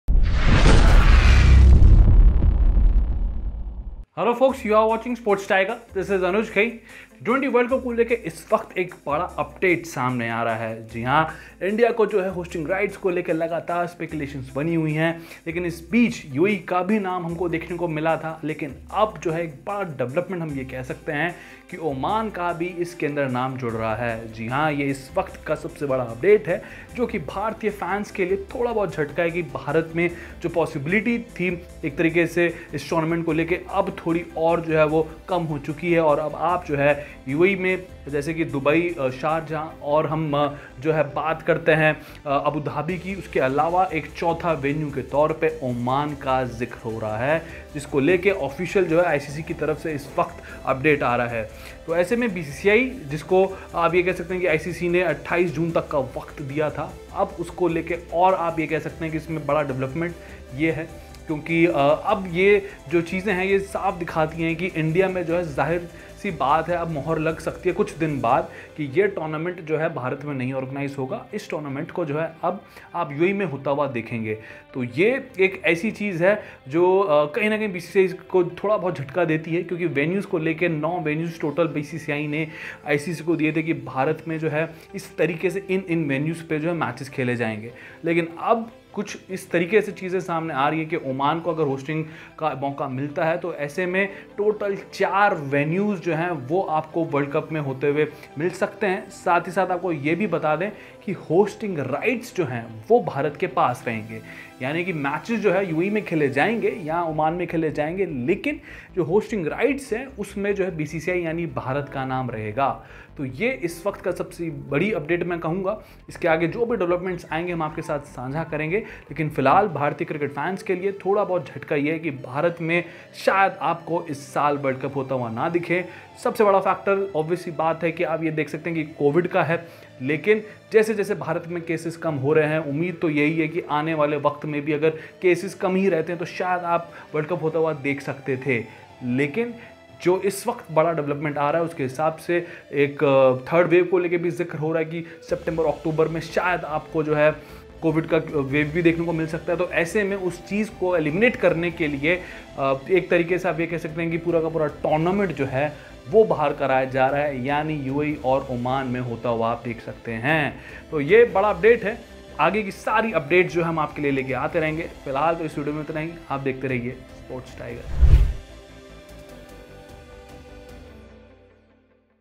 T20 World Cup के लिए ICC और BCCI को मिला नया Venue | जानिए FULL DETAILS
In a recent development, it is being known that International Cricket Council (ICC) and the Board of Control for Cricket in India (BCCI) are in talks with Oman Cricket over hosting of some of the T20 World Cup matches in the gulf nation. Earlier this week, the ICC had given BCCI a month’s time to show its readiness to host the tournament in October-November.

But according to the latest update, the T20 World Cup is all set to be shifted to the UAE and Oman. Oman is one of the 16 participating nations at this year’s T20 World Cup, scheduled in October-November.

Oman’s capital Muscat has been reportedly added as the tentative fourth venue, apart from Abu Dhabi, Dubai, and Sharjah, for the upcoming ICC T20 World Cup. Even if the tournament is shifted out of the country, India will still retain the hosting rights. A top Oman Cricket official has also confirmed that ICC and BCCI are in talks with Oman Cricket over hosting some of the T20 World Cup matches in Oman.